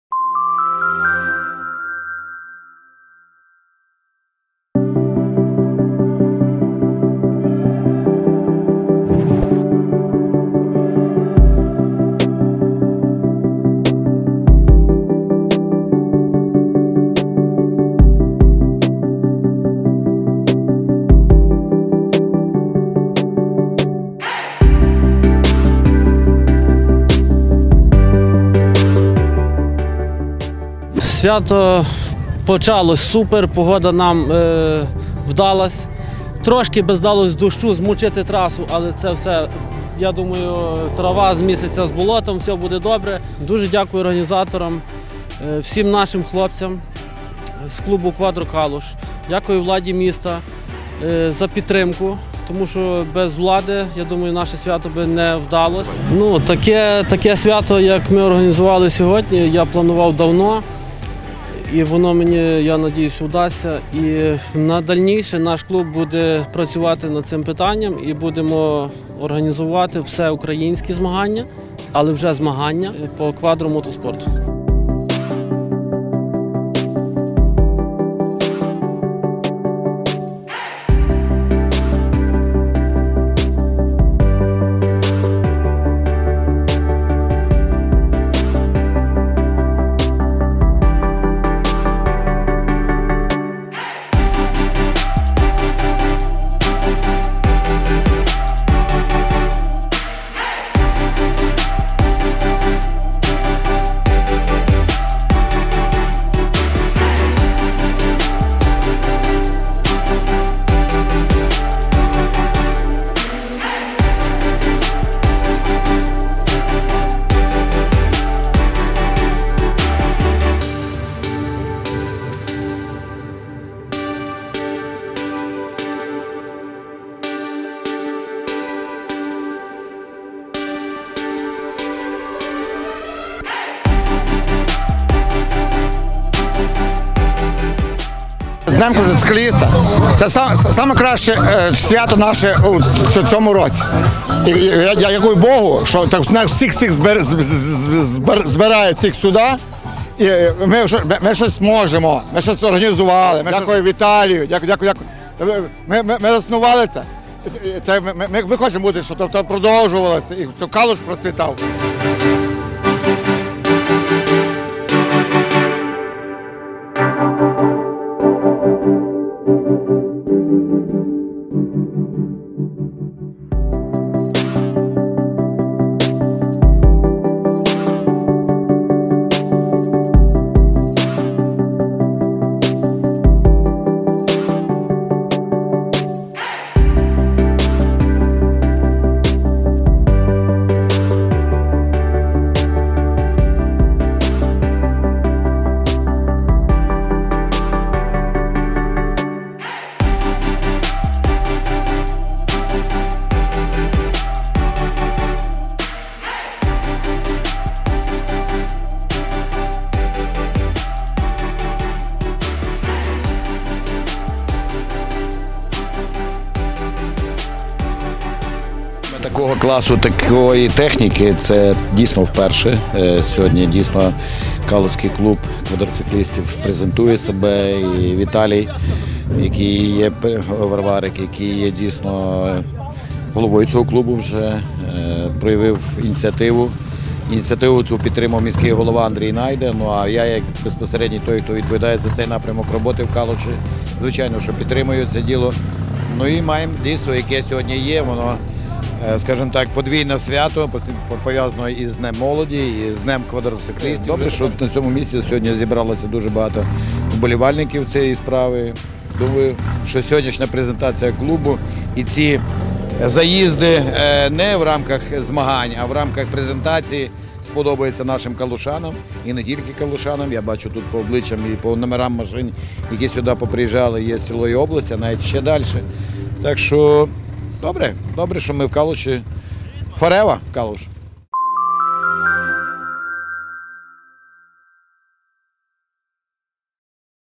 Kvadro_Kalush вразив шоу на квадроциклах та багі
27 червня в Калуші вперше пройшло свято квадроцикліста. Учасники свята проїхалися квадро-мотоколоною центральними вулицями міста і зупинилися біля Каменя Незалежності, де й відбулося урочисте відкриття заходу. Потужний гуркіт двигунів та прапори із логотипом клубу «Kvadro_Kalush» зацікавили багатьох калушан, а особливо дітей, які охоче знайомилися з технікою і робили фото на пам'ять. Проте родзинкою свята були показові заїзди квадроциклістів та їхніх колег в районі мотополігону в Підгірках. Саме бездоріжжя — це стихія квадроциклів та багі.